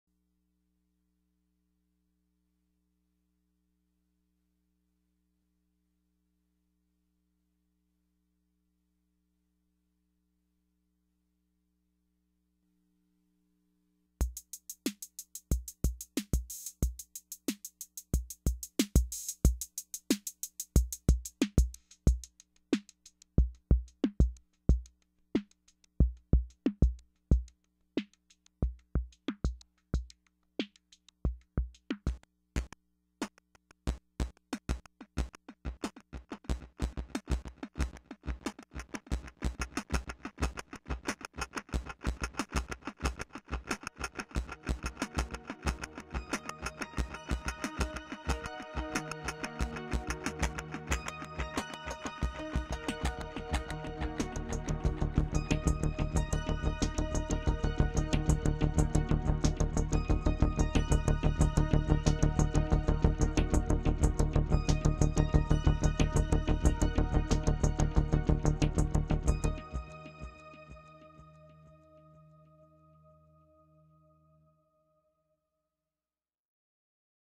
Roland System 1M Audio External Audio Input Demo
This was WAY more complicated than what I wanted it to be. I was thinking it'd be cool to run a drum machine through the Sys1M's filter and FX. The Sys1M does not have an envelope follower, so it has to be triggered. I run 16th note trigger pulses from the 606 to the 101's external sync. The 101's CV&Gate are run into the Sys1M to trigger the unit and send CV to the filter cutoff. Added a little Jupe 4 through Strymon BigSky and some SH-2 bass.